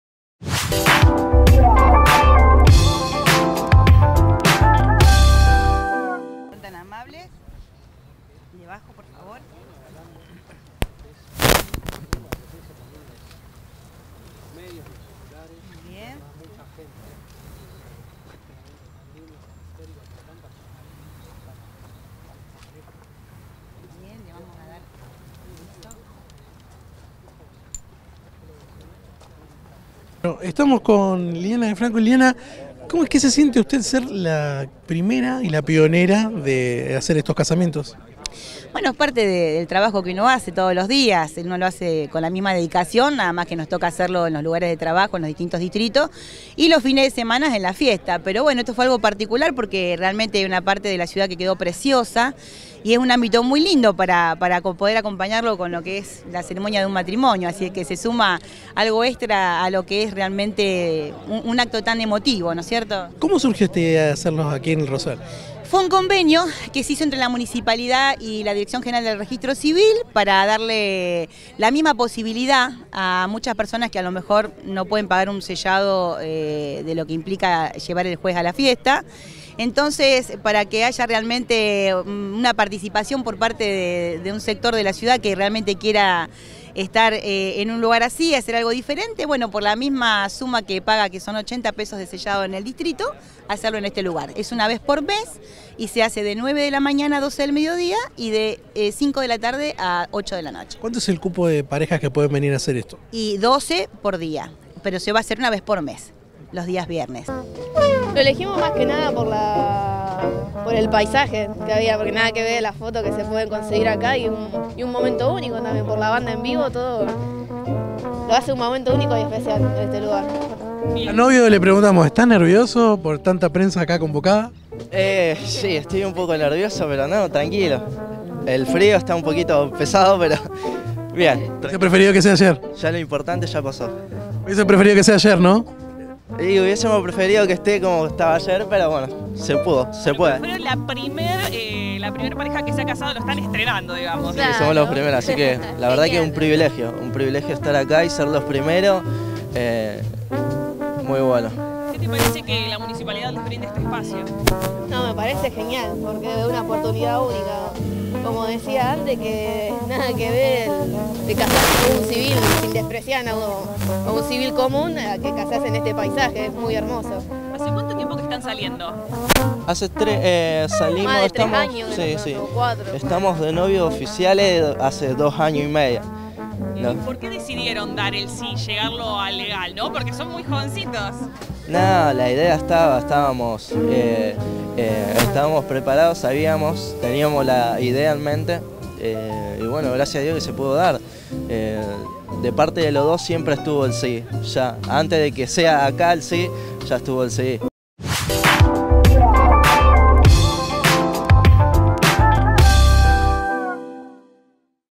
Matrimonios dan el “sí” en el Rosedal del Parque Independencia
Se debe a una iniciativa impulsada por el municipio en conjunto con el gobierno de la Provincia y el Registro Civil. A pesar de la fría mañana, las parejas levantaron felices su libreta al aire libre.